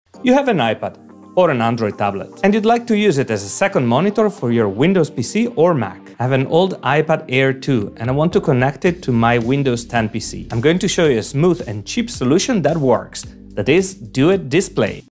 Wow! iPad as second monitor with Duet Display
Duet Display is probably the best app to use your iPad as second screen. It used to have high latency in the past, but in February 2022 a new version was released and now there's almost no lag.

duet display